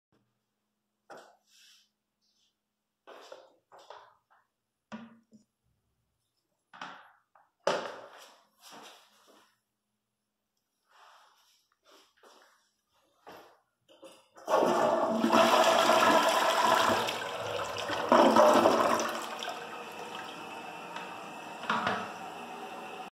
1998 Toto Carusoe Toilet with a Big Trapway at Comfort Inn (Lenoir, NC)
No, I'm not staying here. I just paid a visit to this place since it was near a Goodwill I went by.

This toilet is ok. I like this one since the trapway is wide, but the flush could've been a lot better. This might be original to the place.

Rating: 7/10